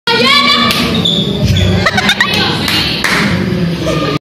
Niño leda una galleta a otro en el Pedro Enrique ureña
Niño leda una galleta a otro en el
Pedro Enrique Ureña  23 de setiembre
del mes 9 del 2015.